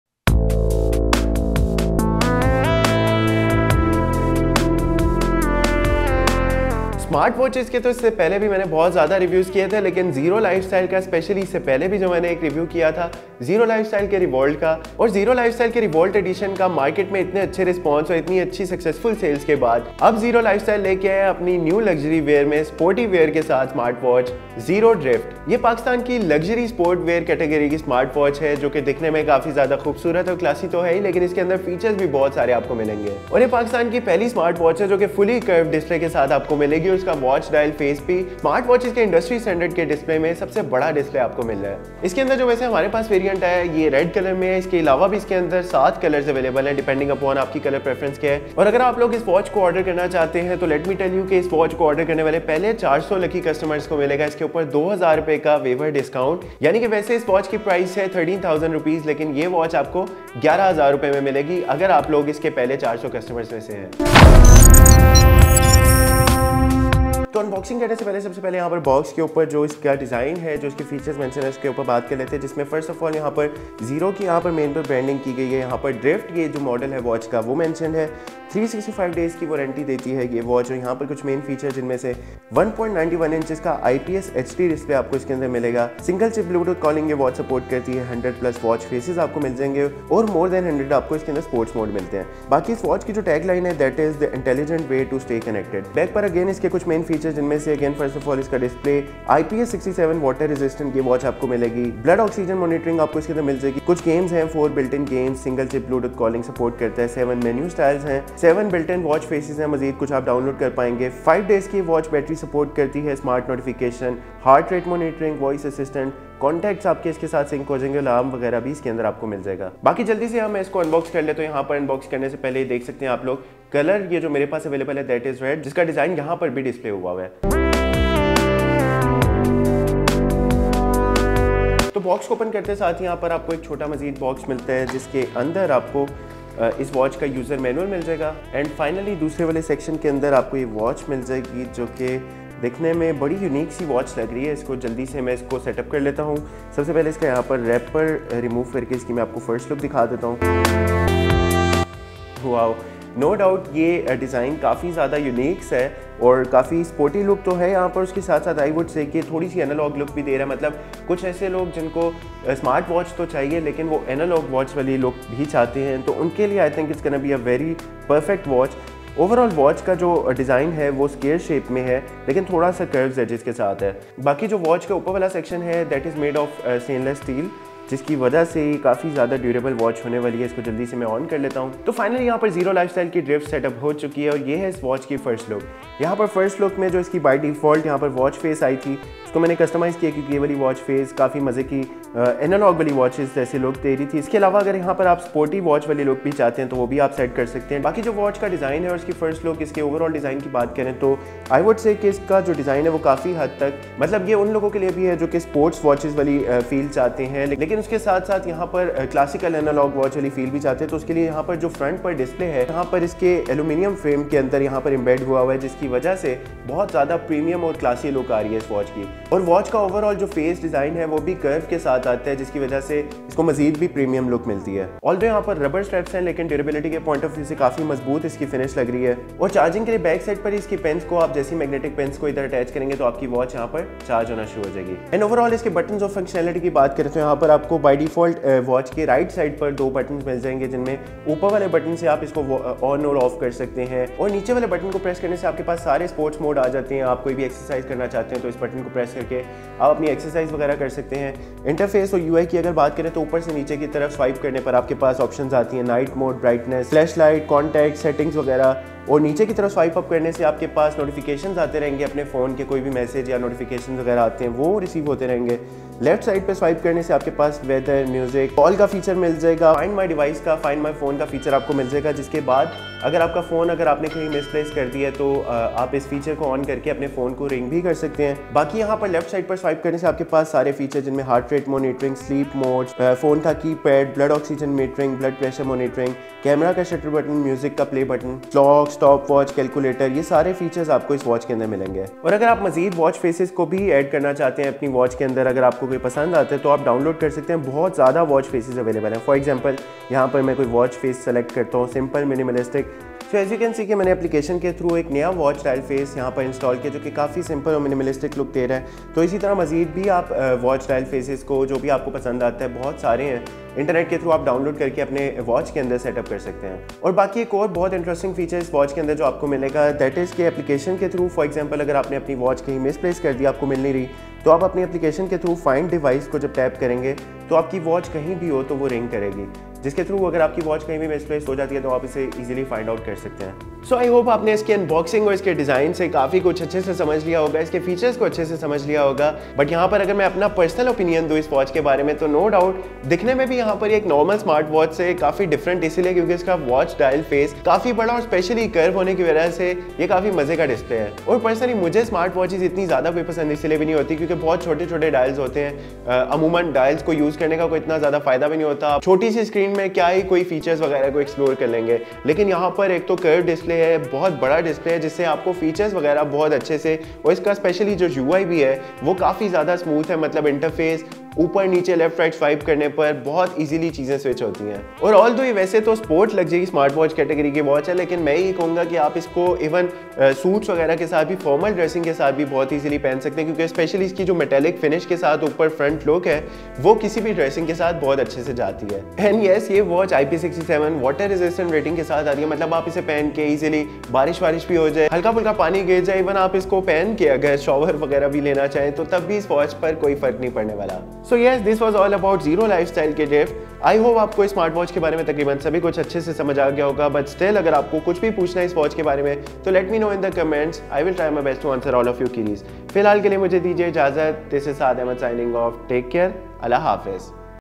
Pakistan's Luxury-Sport Smartwatch by Zero Lifestyle | Drift Luxury Edition II | Style & Features
Welcome to our comprehensive review of the Zero Lifestyle Smartwatch Drift, Pakistan's own Luxury-Sport wear Smartwatch
In this video, we'll take a close look at the Drift's design, features, performance, and overall value, helping you make an informed decision on whether this smartwatch suits your lifestyle and tech needs.

🕒 Timestamps:
0:00 - Introduction
1:10 - Unboxing
2:40 - Design
4:15 - Functions and UI
5:35 - Final Verdict

The Drift is designed to combine style and functionality seamlessly, offering impressive features such as health tracking, customizable watch faces, and connectivity with your smartphone. We'll delve into its fitness tracking capabilities, elegant design, and how it stacks up against other popular smartwatches.

Make sure to stick around until the end for our final verdict and recommendations on whether the Zero Lifestyle Smartwatch Drift is the right choice for you.